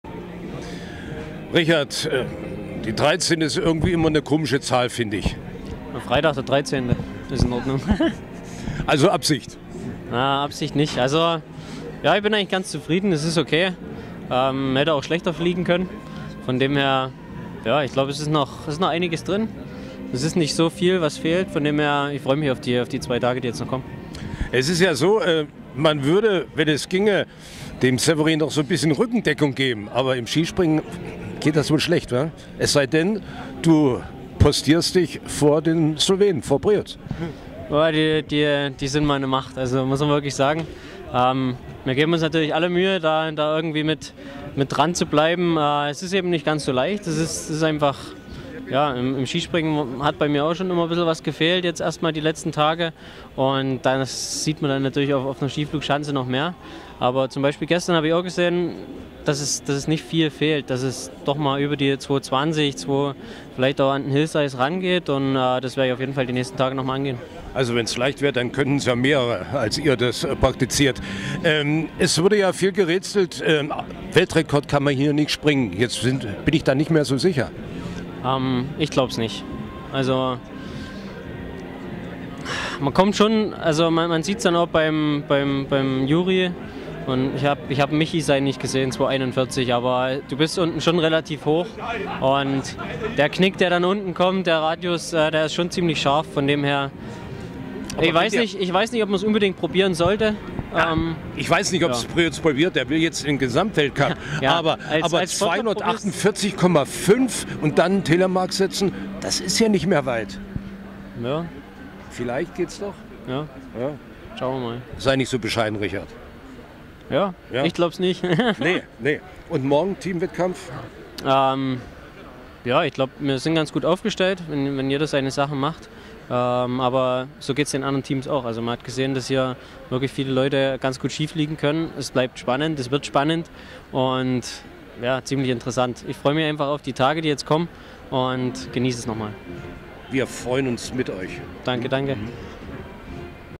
Richard Freitag im Interview - Planica 20.03.2015
Dirk Thiele und Gerd Siegmund berichten vom Weltcupfinale im Skispringen aus Planica.